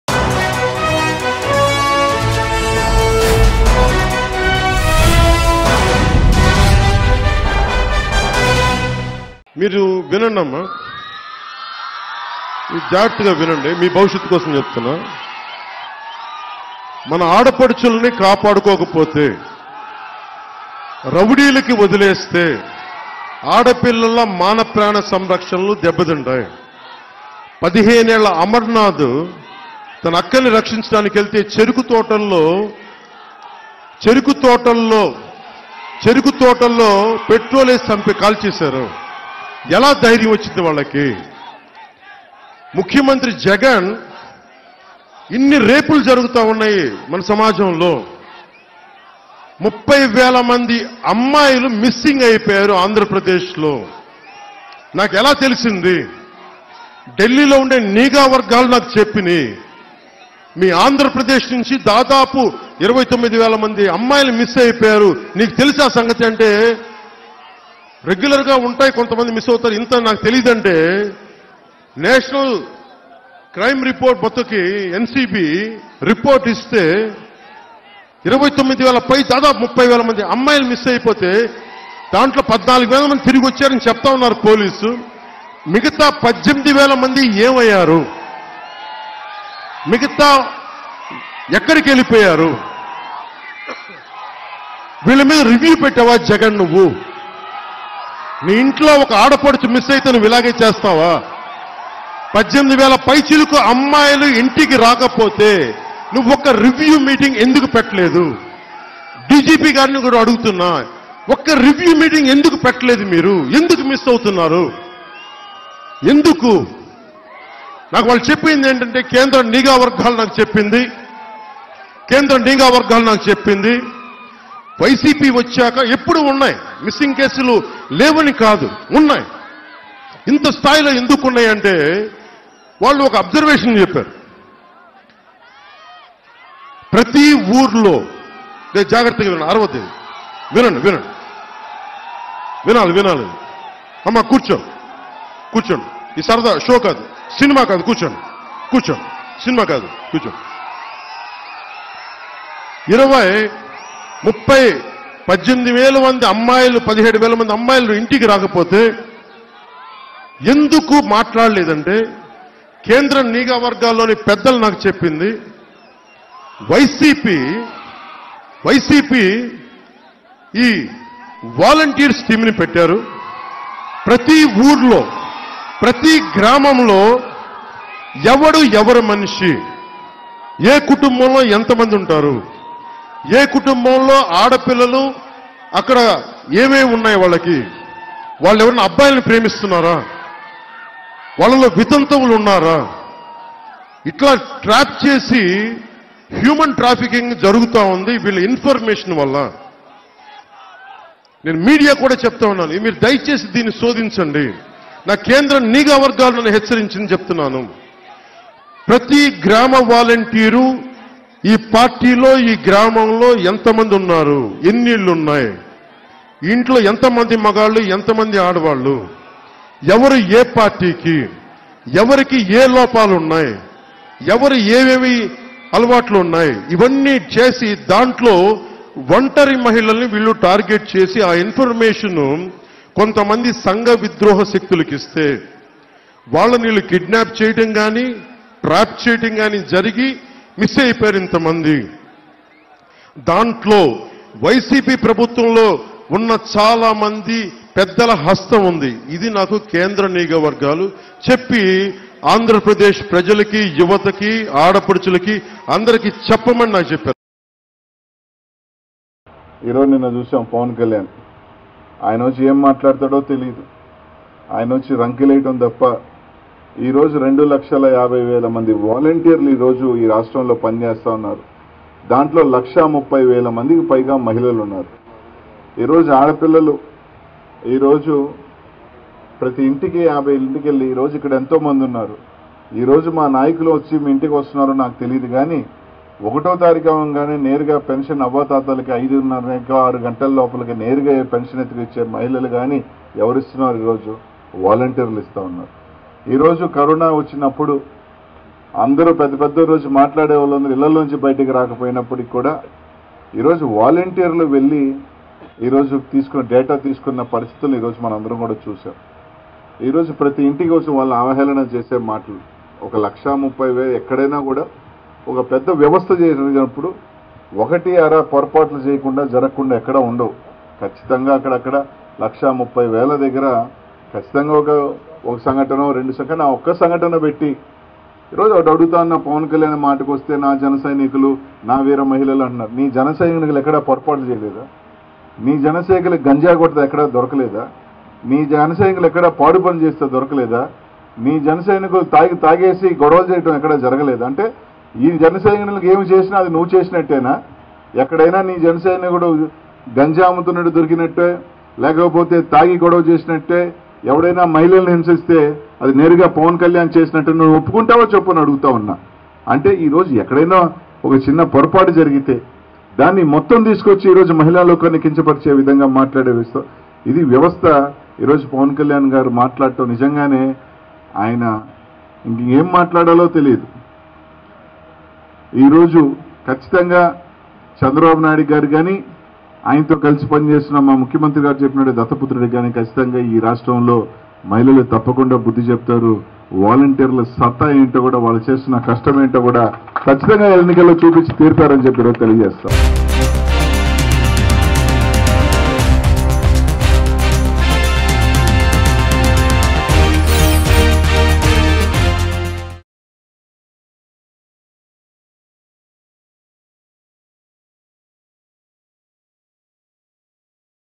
Action And Reaction: Pawan Kalyan Vs Anil Kumar Yadav On AP Volunteers | AP Politics | Mango News
Action And Reaction: Pawan Kalyan Vs Anil Kumar Yadav On AP Volunteers.